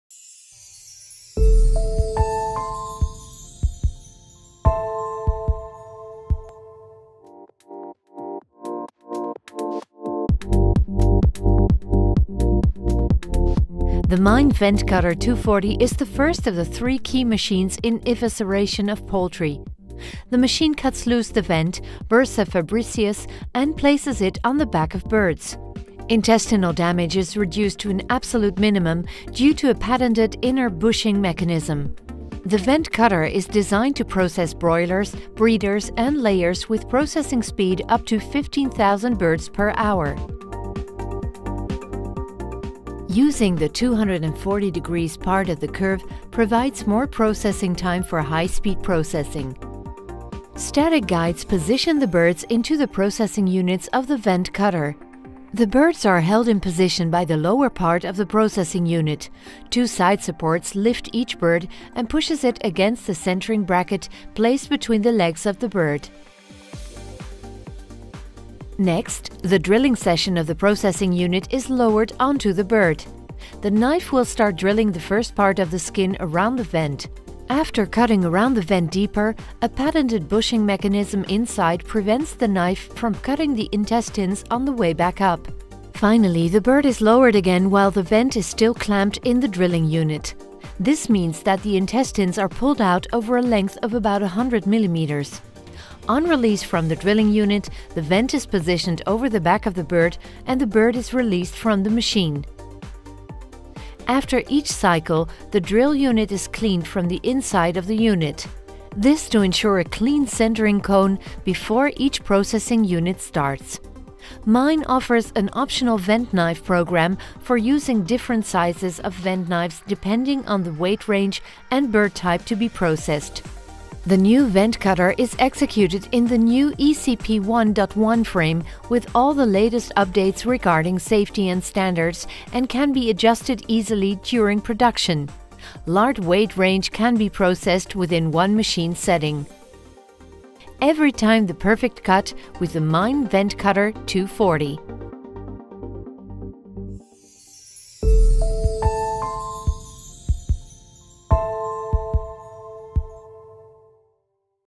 Meyn® Vent cutter 240
The Vent cutter 240 is the first of the three key machines in poultry evisceration, designed to process broilers, breeders, and layers. It cuts loose the vent (cloaca), bursa of Fabricius, and places it on the back of birds.